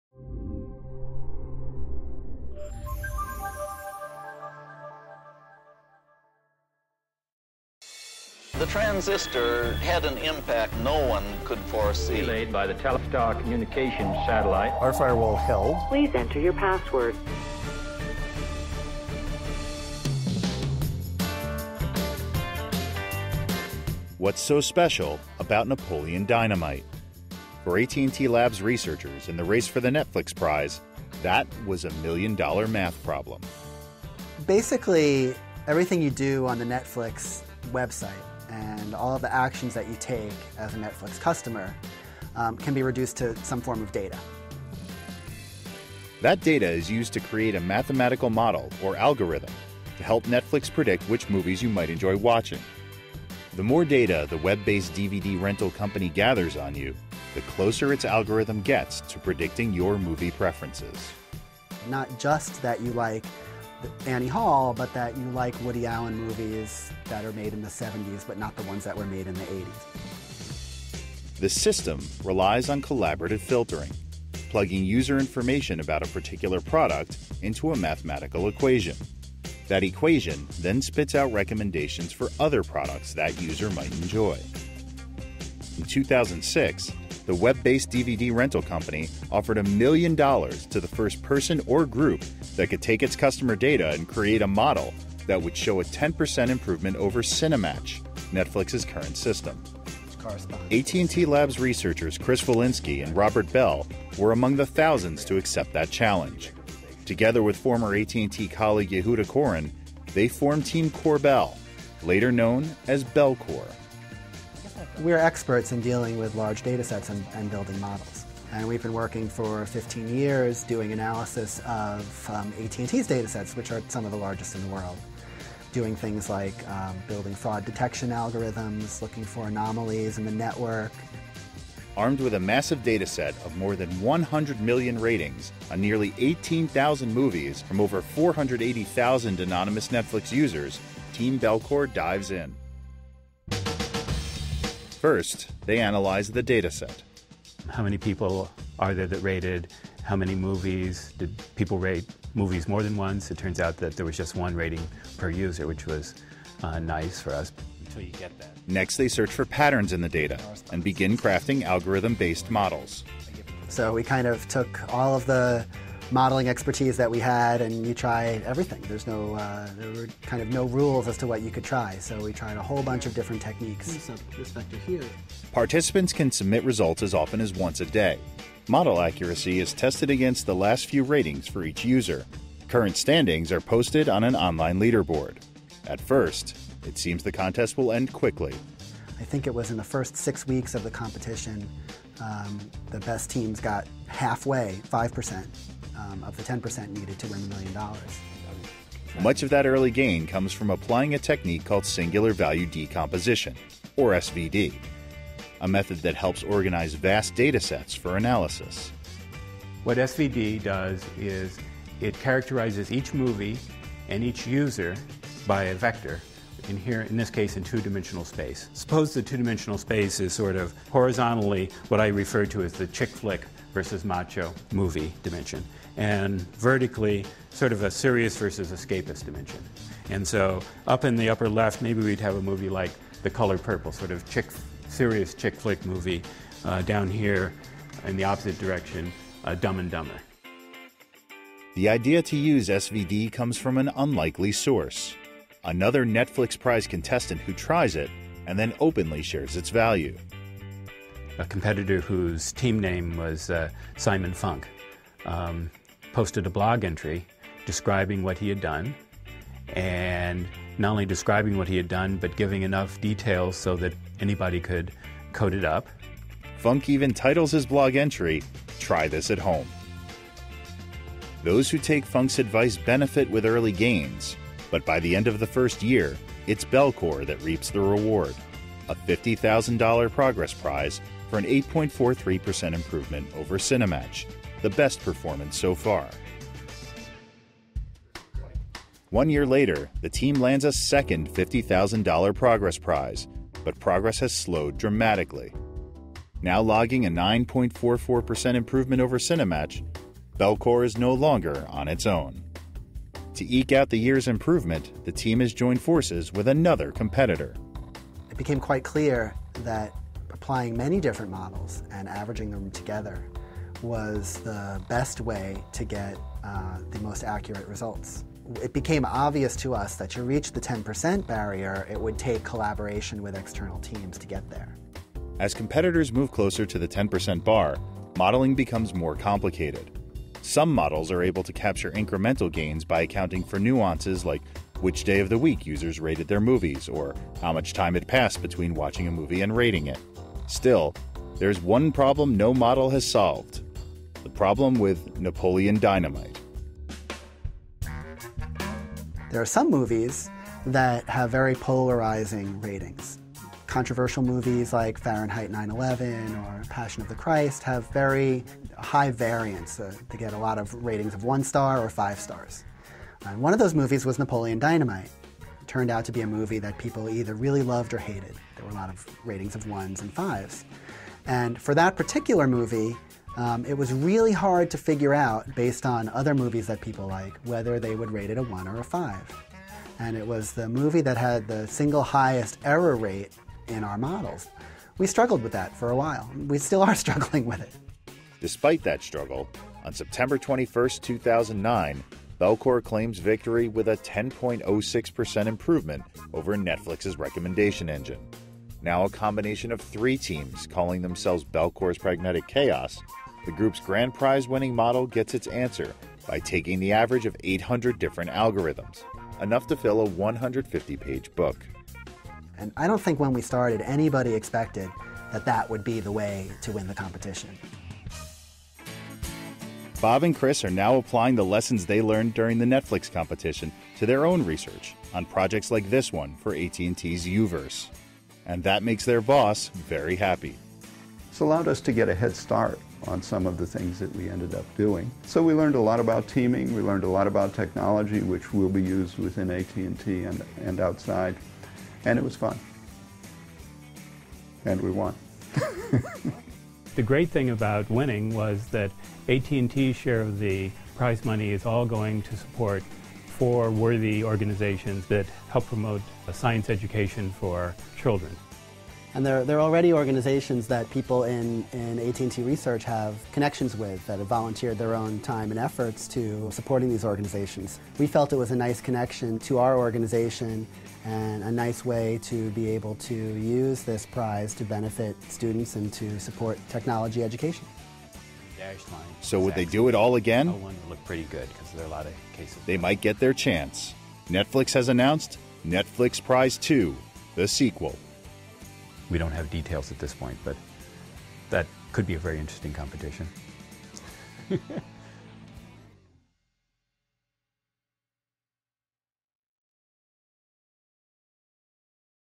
From the Labs: Winning the Netflix Prize
AT&T Labs researchers and million dollar Netflix Prize co-winners, Chris Volinsky and Robert Bell, describe their three-year quest to improve the collaborative filtering algorithm Netflix relies on to make millions of movie recommendations.